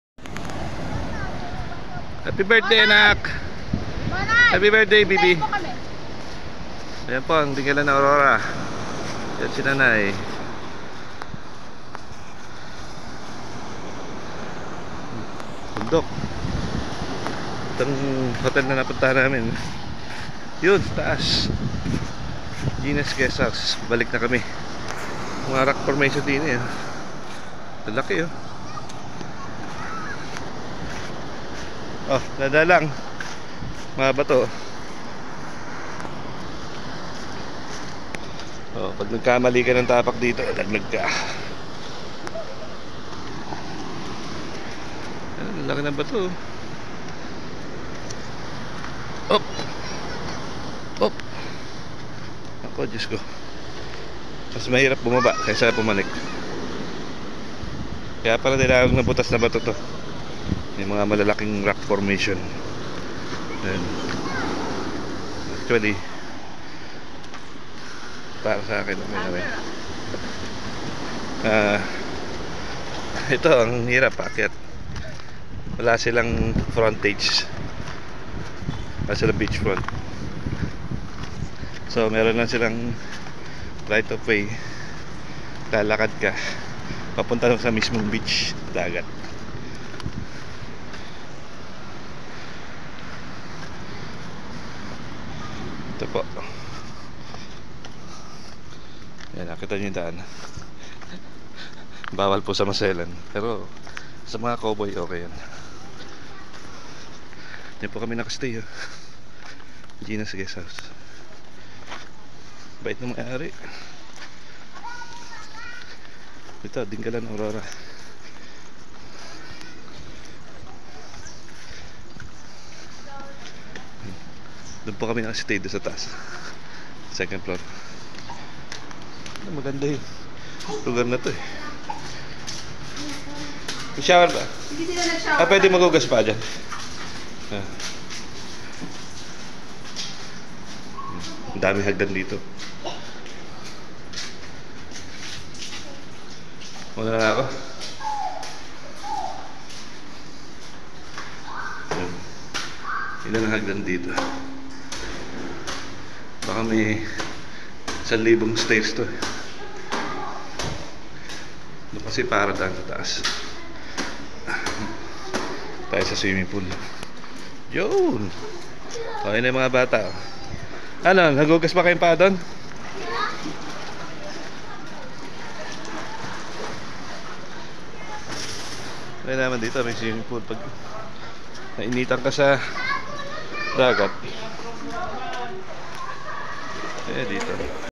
Dingalan Aurora walk
Hi! Good morning po sa inyong lahat. Share lang po namin our trip to Dingalan, Aurora, na hindi naman po kalayuan sa Cabanatuan City, Nueva Ecija. About 1.5 hours lang po siguro byahe from Cab City. Birthday po ng anak namin si Athena Amarah, so napagpasyahan po namin na dito magpunta sa Dingalan. Sa Gina's Guest House po kami nagpabook and ok naman po yung place, maganda, highly recommended po. February 08, 2023 po pero to our surprise, maulan po sa dingalan kahit po sa Laur at Gabaldon pa lang siguro na rin po dahil Type 2 climate po dahil nasa eastern Luzon po meaning, pag tag=araw sa western side aka Zambales Manila, maulan naman po sa east coasts like Cagayan Isabela and Aurora. Thanks for viewing po.